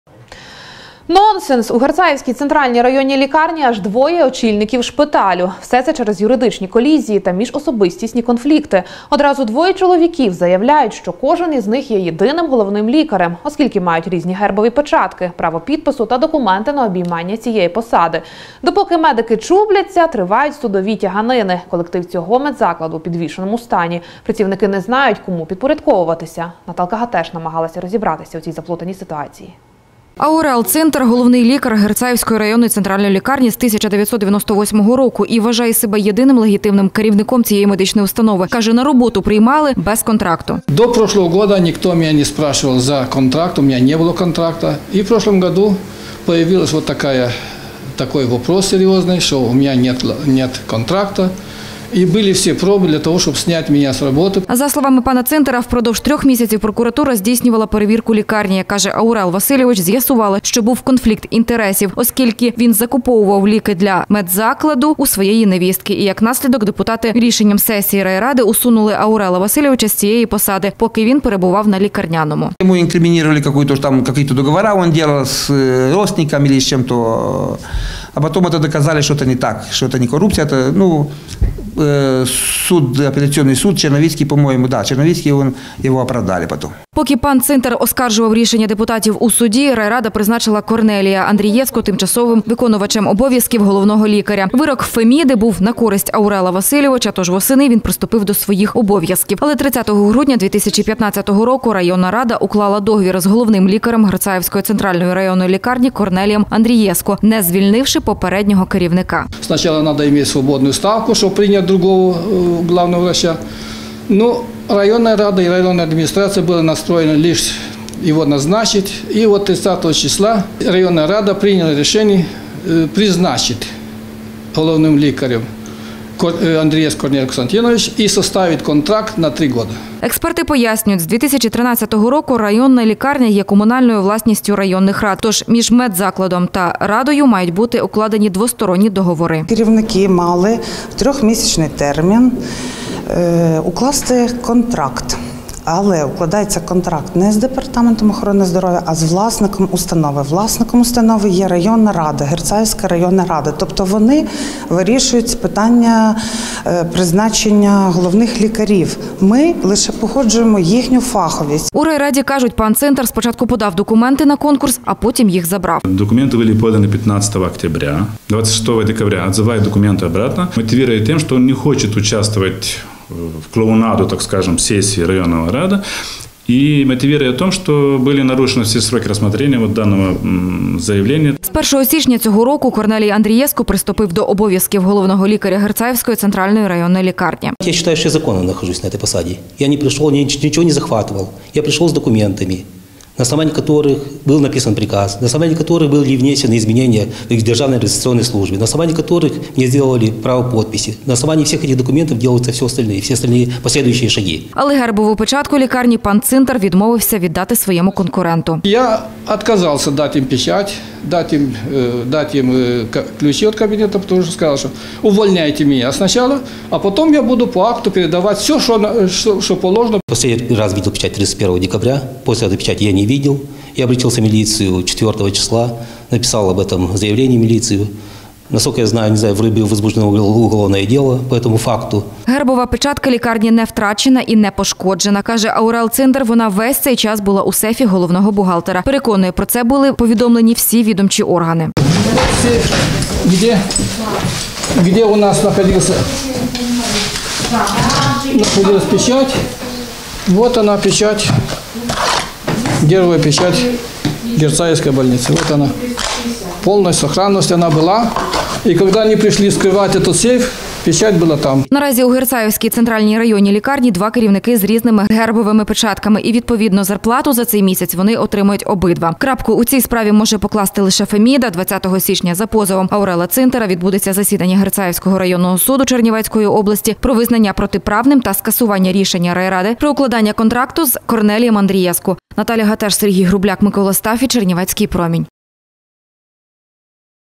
Двоє на одному стільці
160119 У Герцаївській центральній районній лікарні аж двоє очільників шпиталю. Усе це через юридичні колізії та, звичайно, міжособистісні конфлікти.